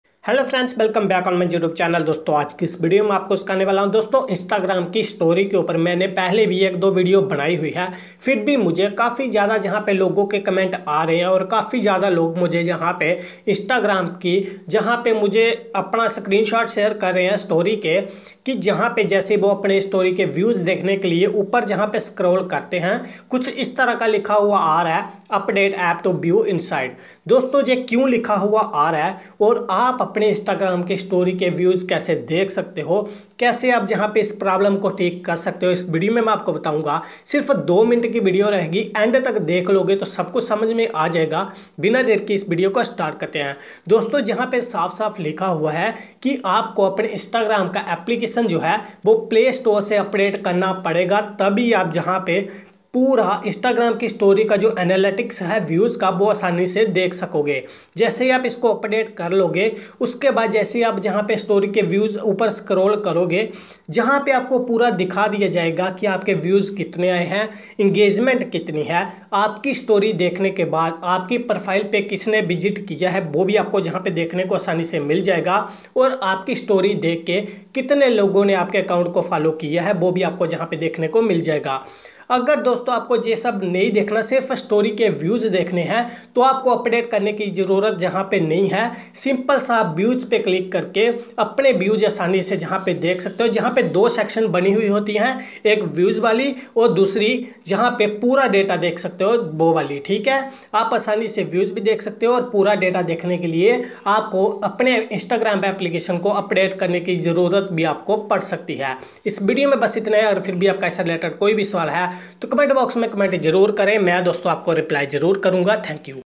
update app to view insights on instagram
Hello friends,
Is video me mai apko batane wala hoon ki agar apke instagram me b update app to view insights aa raha hai toh aap kaise thik kar saktey hai.